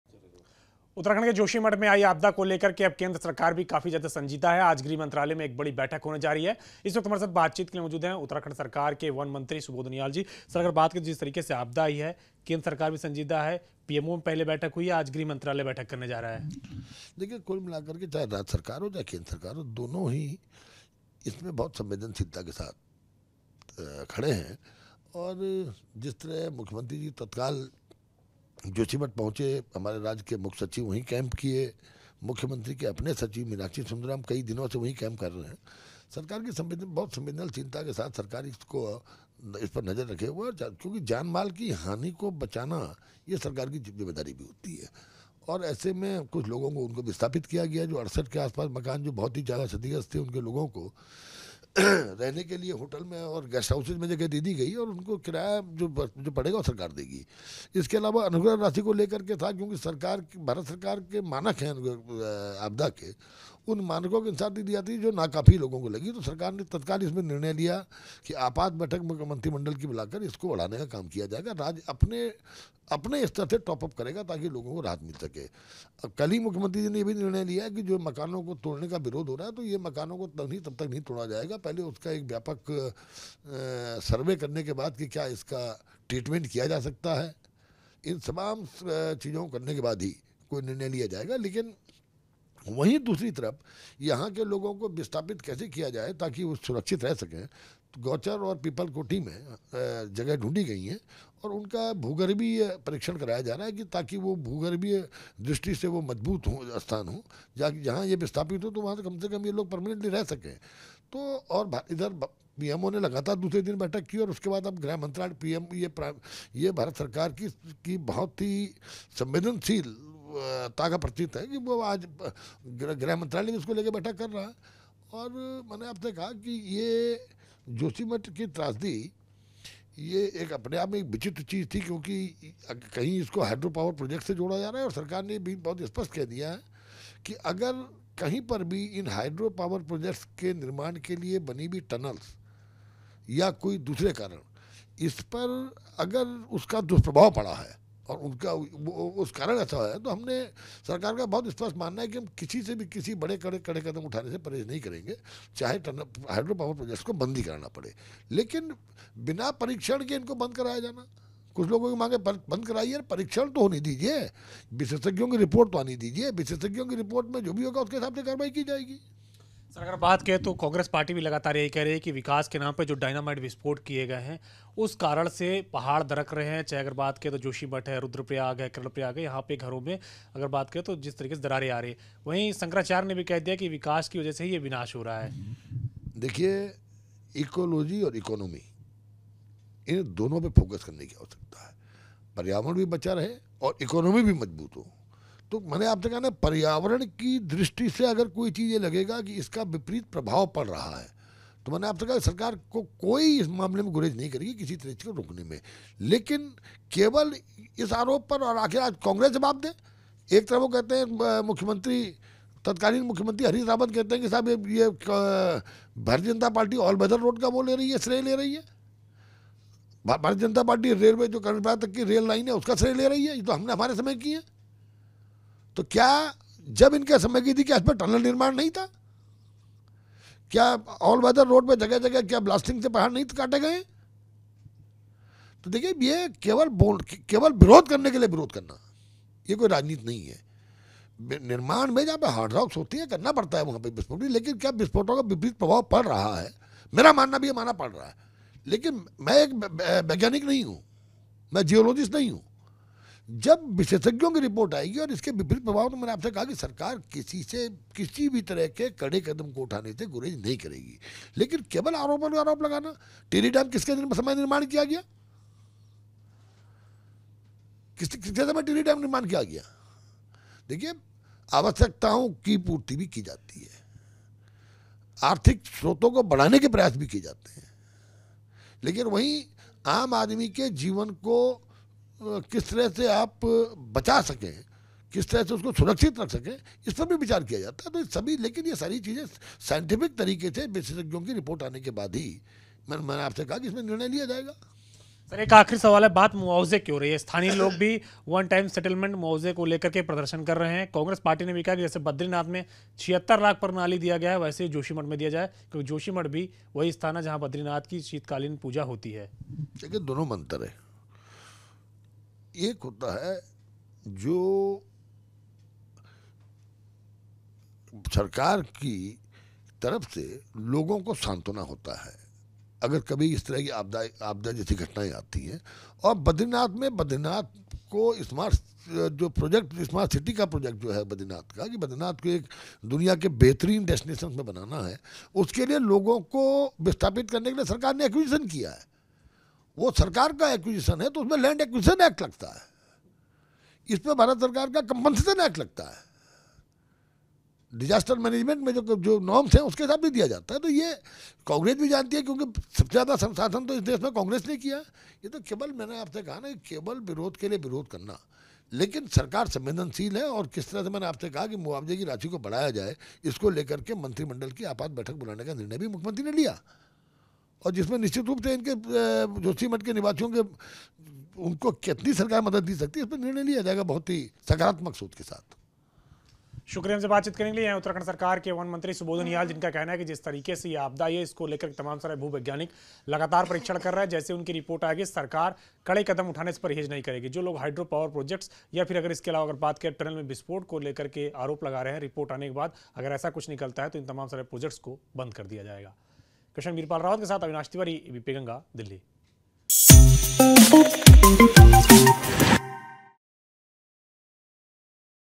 बद्रीनाथ की तरह ही जोशीमठ को मुआवजा दिया जाए ये संभव नहीं : Subodh Uniyal
उत्तराखंड सरकार में वन मंत्री सुबोध उनियाल ने कहा केंद्र सरकार लगातार संजीदा है पहले पीएमओ में बैठक हुई और अब अगर बात करे तो गृह मंत्री ने बड़ी बैठक करी है जो भी मदद हो सकती है सब की जा रही है. सीएम से लेकर कई मंत्री अधिकारी जोशीमठ में कैम्प कर रहे है लिहाजा कोई ये ना कहे कि सरकार संवेदनशील नहीं है. भारत सरकार के आपदा को लेकर मानक है लिहाजा उंस मानक से ज्यादा मुआवजा देने के लिए राज्य सरकार काम कर रही है. वैज्ञानिक लगे है अगर वैज्ञानिक बताते है विस्फोटक या टनल निर्माण की वजह से ऐसा है तो ततकाल सारे विकास कार्य बंद कर दिए जाएंगे. बद्रीनाथ में विकास के लिए भूमि अधिग्रहण कानून भी जमीन के लिये लागू हुआ जबकि जोशीमठ में आपदा कानून लागू होता है लिहाजा कांग्रेस पार्टी ये न कहे बद्रीनाथ की तरह ही जोशीमठ को मुआवजा दिया जाए ये संभव नही।।